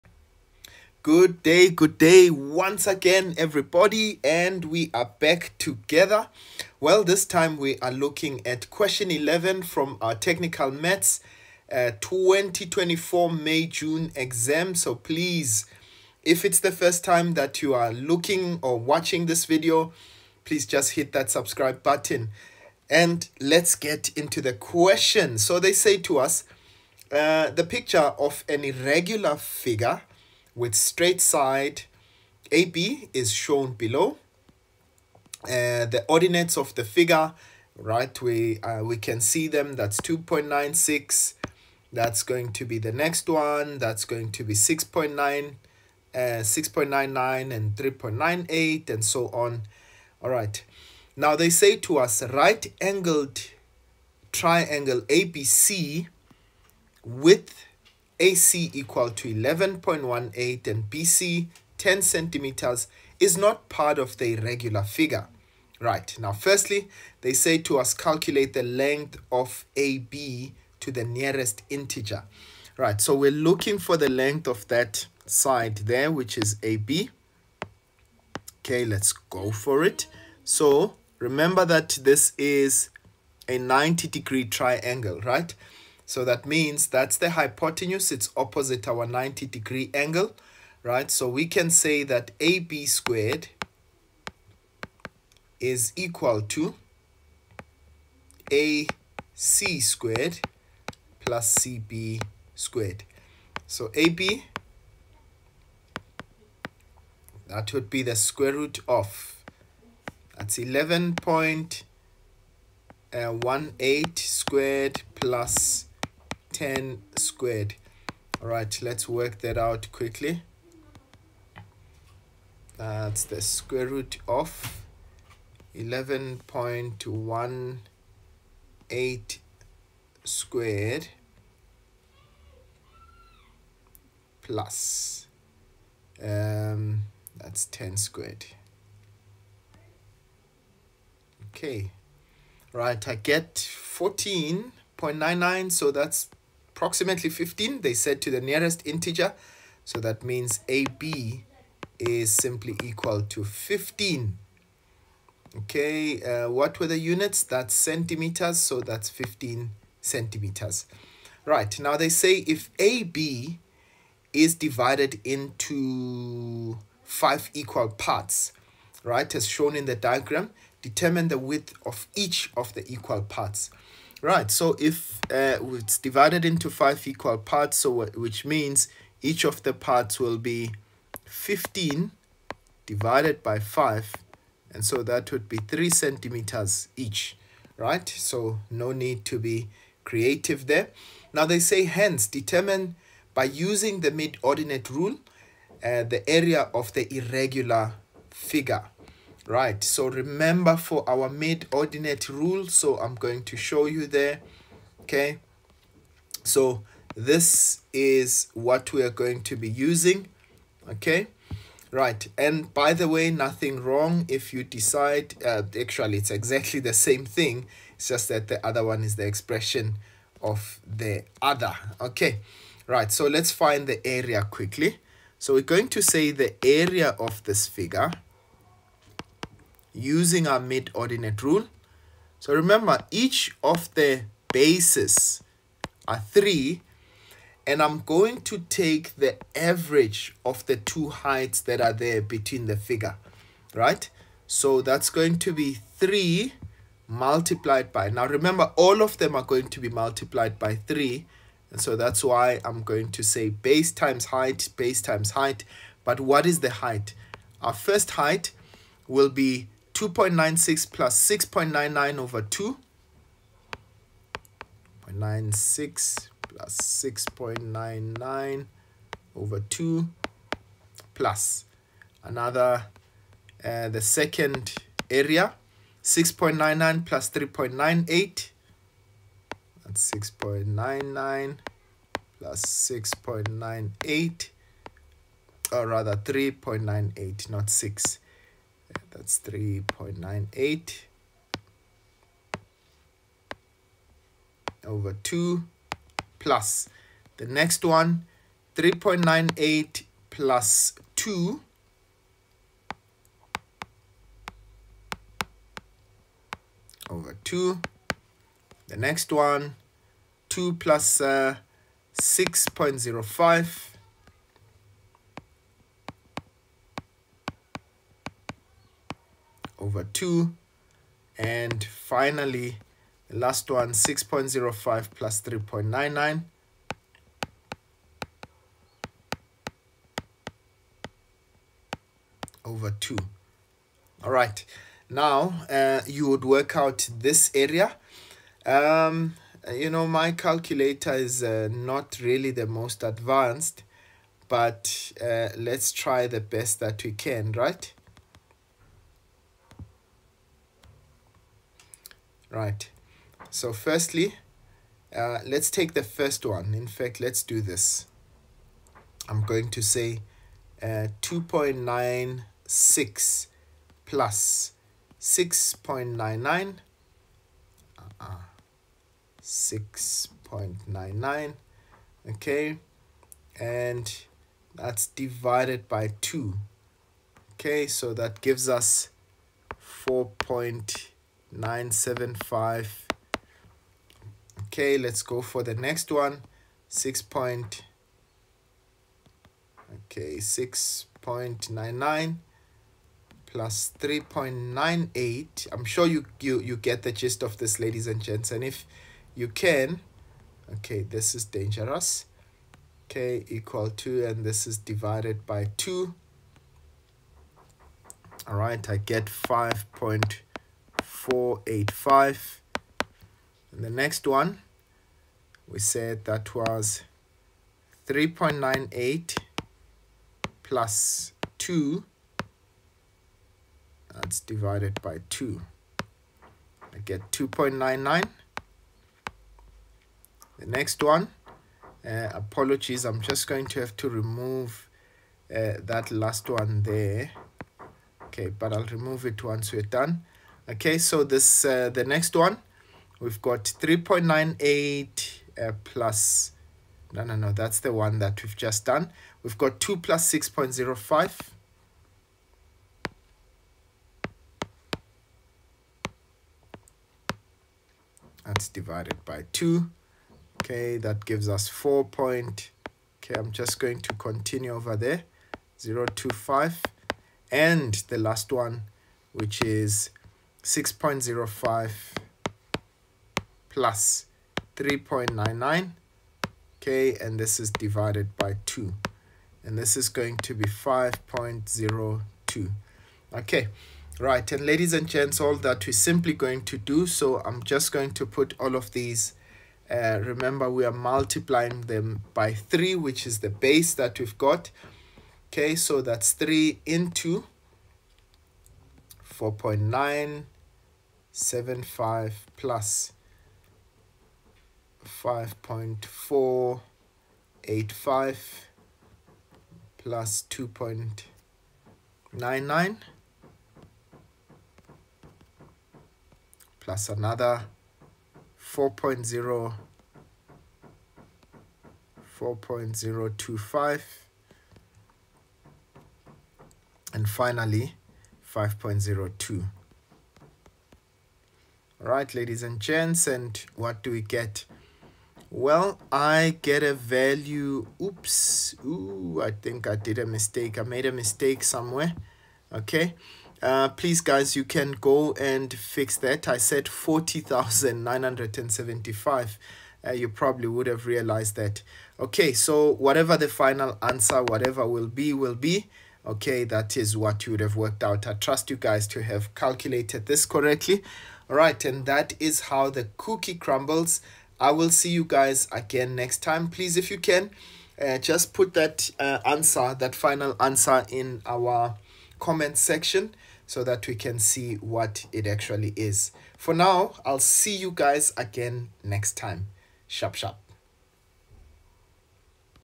Calculating the Area of an Irregular Figure Using the Mid-Ordinate Rule | Grade 12 Tech Maths
Grade 12 Tech Maths students — struggling with irregular figures? This lesson breaks down how to calculate the area of an irregular shape using the mid-ordinate rule.

We take you through a step-by-step process, showing you how to apply the rule correctly, avoid common mistakes, and ace exam-style questions.

📌 In this video:

Understanding the mid-ordinate rule

Applying it to irregular figures

Step-by-step worked example

Common errors to avoid in exams

🎓 Mlungisi Nkosi Maths & Science Centre